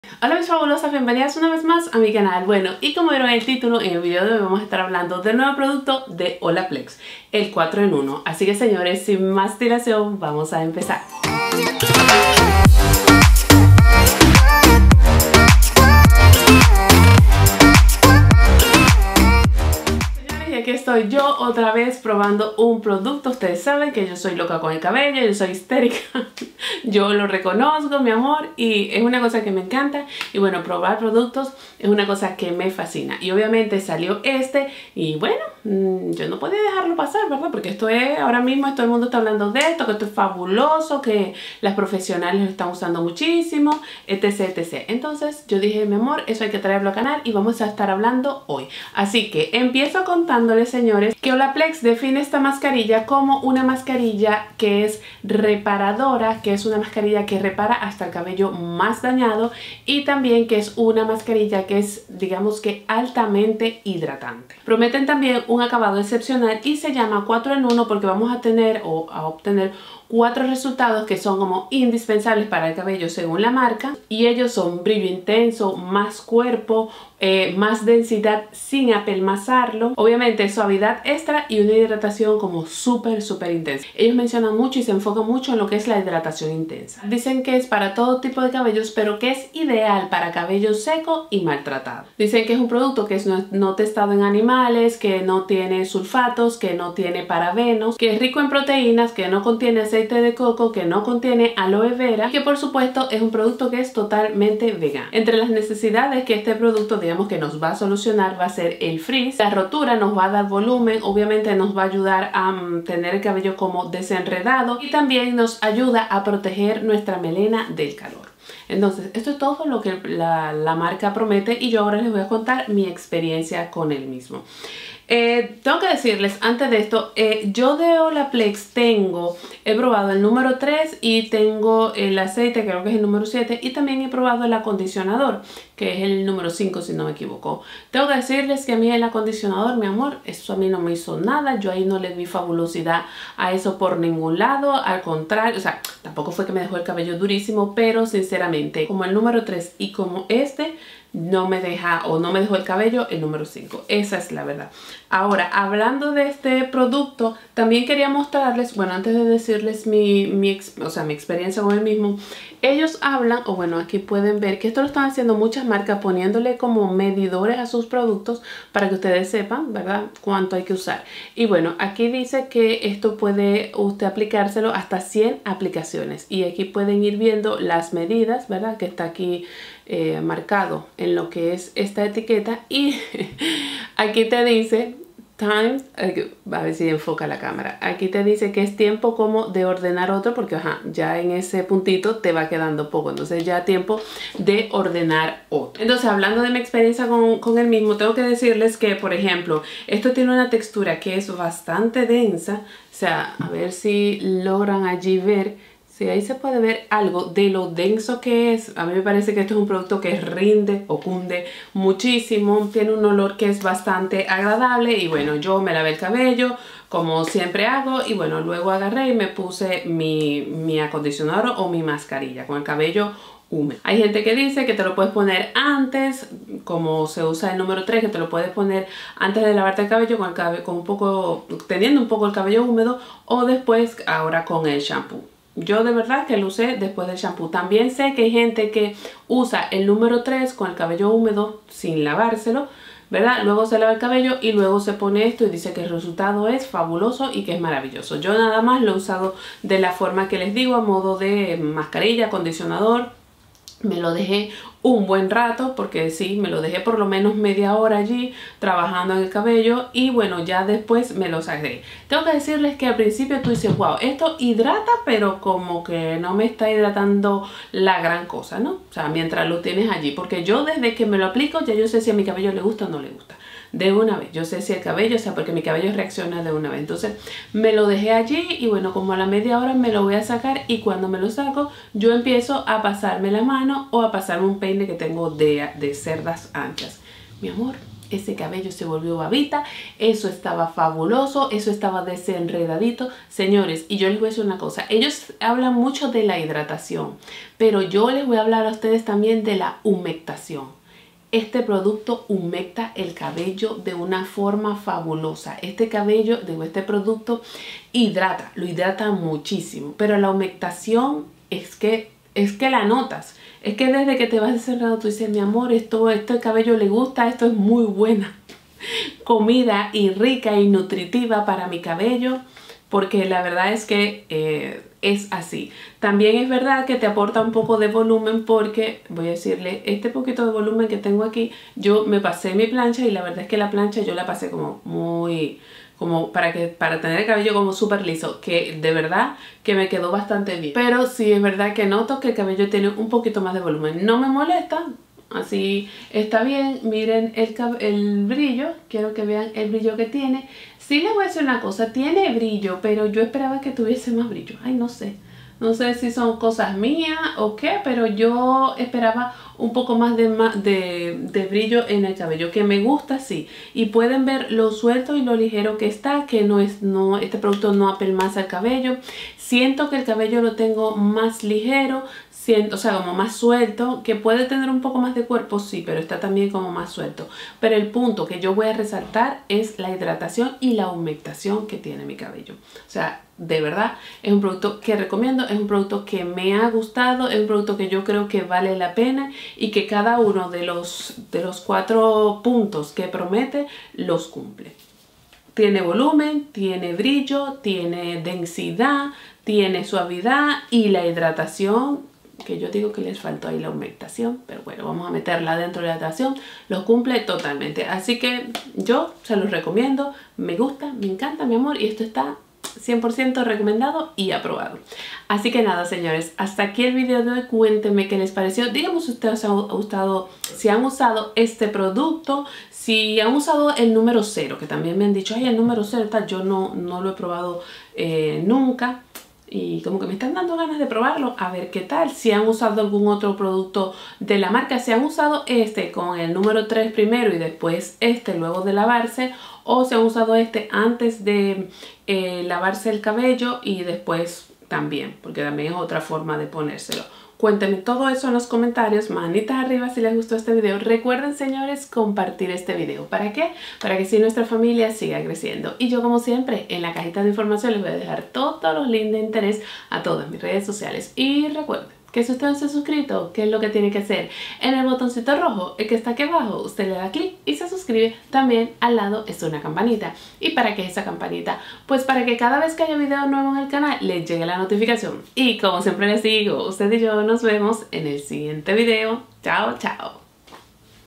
OLAPLEX 4 EN 1 | REVIEW | NINA
Mis fabs hoy hablamos de @Olaplex y su nuevo producto 4 en 1.

ADDRESS
Ninoska
620 Centerpoint Blvd
United States

VIDEOS RECOMENDADOS.